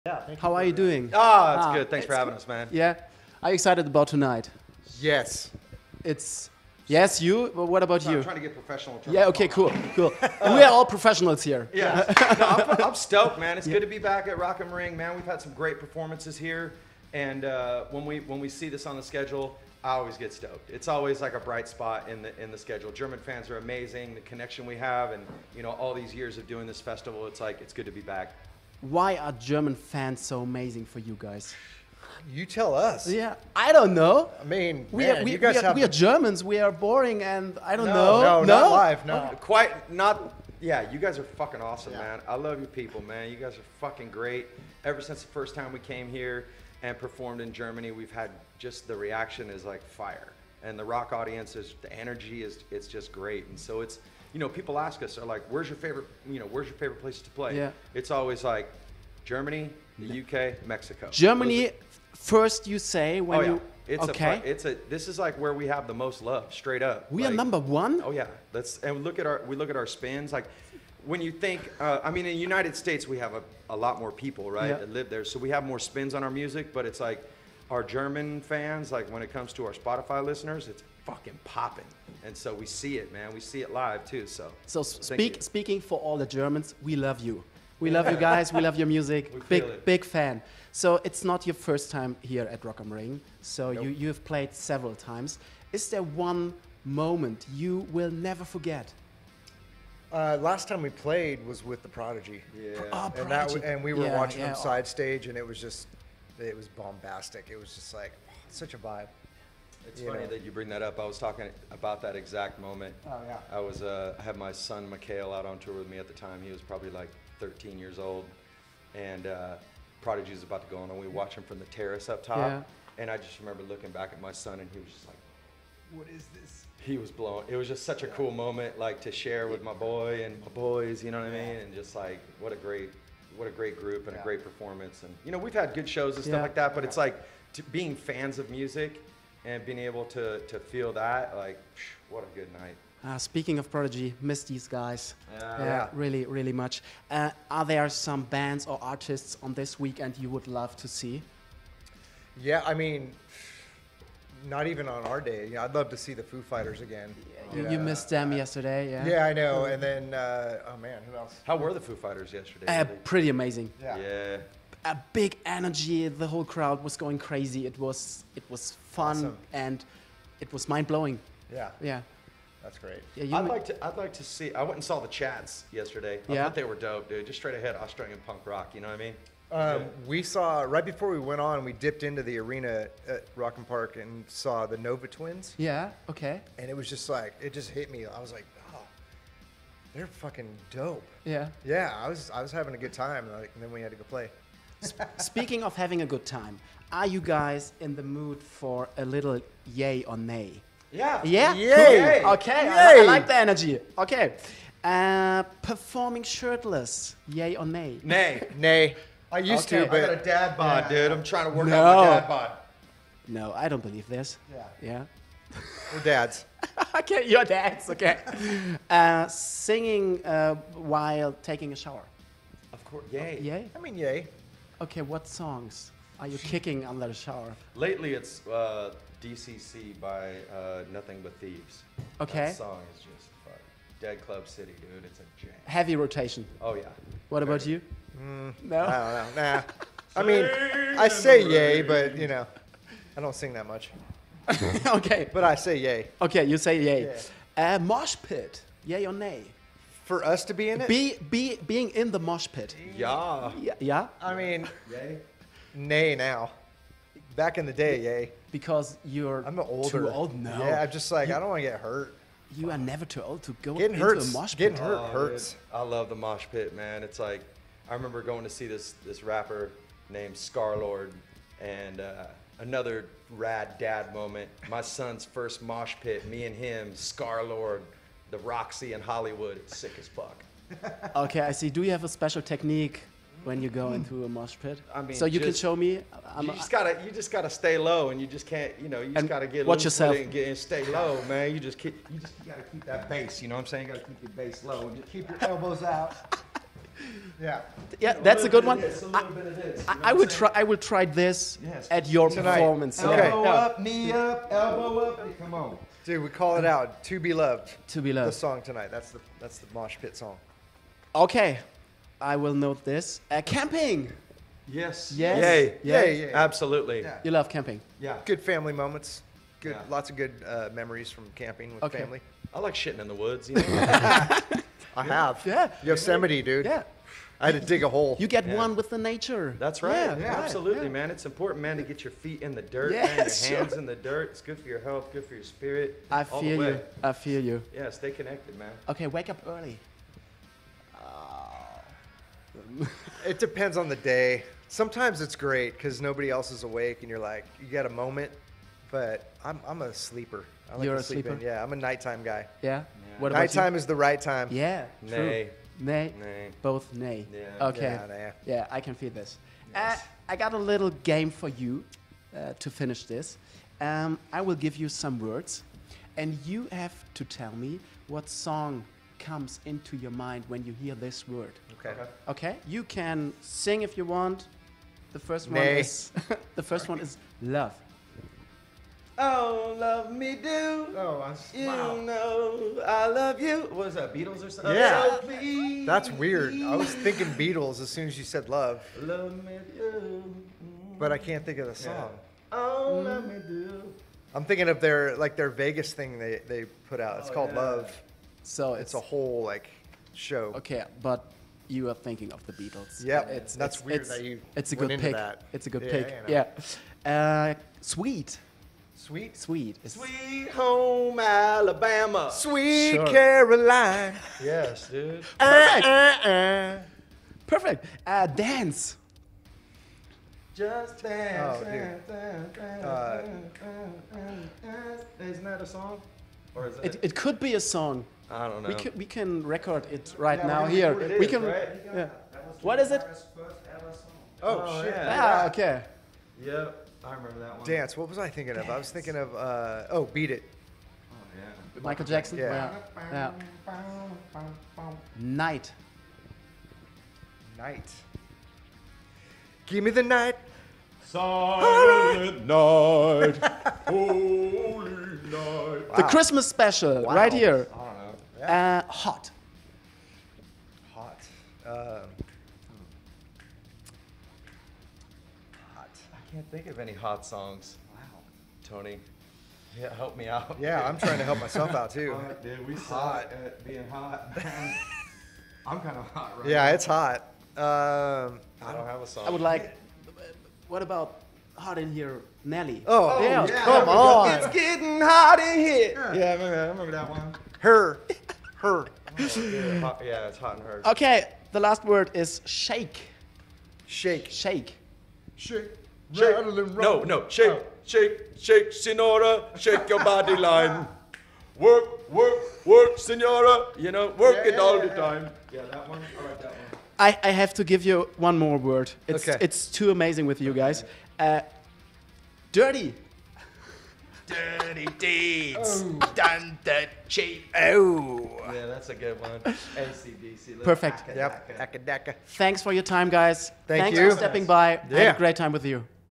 Song Association mit Papa Roach | Rock Am Ring
Den Auftritt von Papa Roach von Rock Am Ring 2023 könnt ihr jetzt auf RTL+: nachschauen